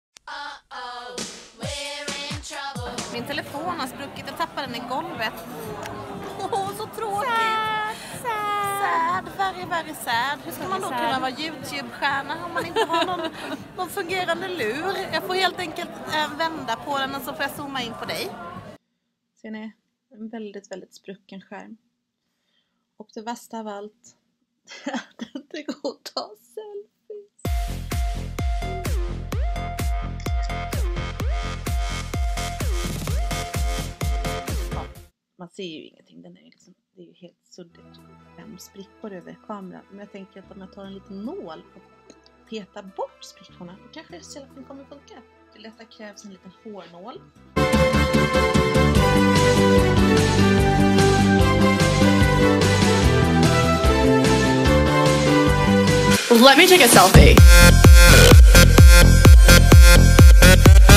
Apan lagar en sprucken iphoneskärm
Råkade tappa mobilen med skärmen nedåt i en stenplatta. Den dog. Nu lagar jag den tillfällig.